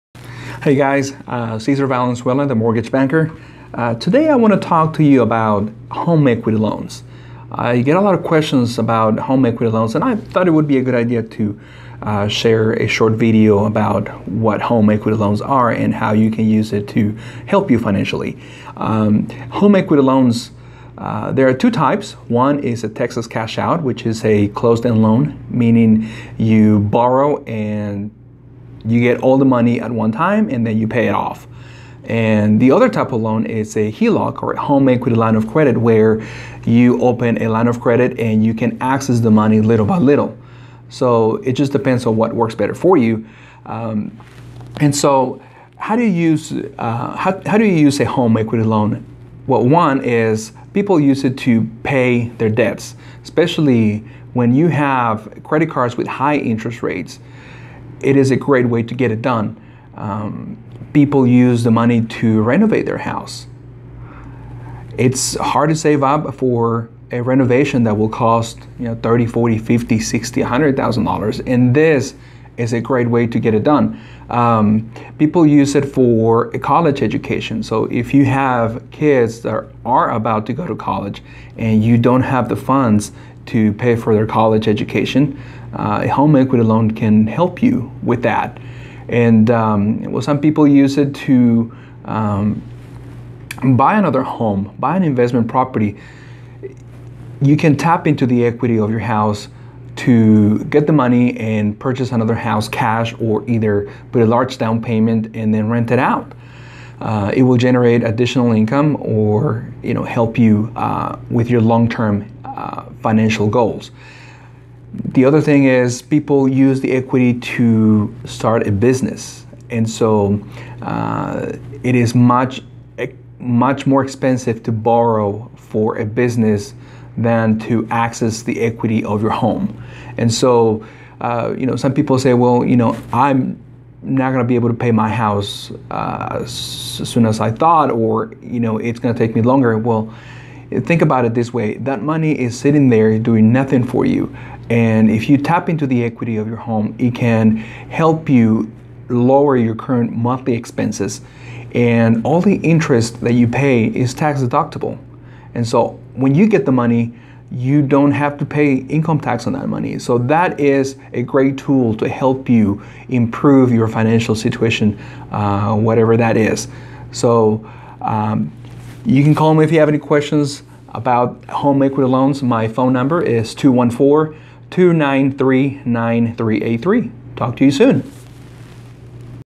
What you need to know about a cash-out home equity loan.
Cesar Valenzuela The Mortgage Banker,  what you need to know about using home equity in your home.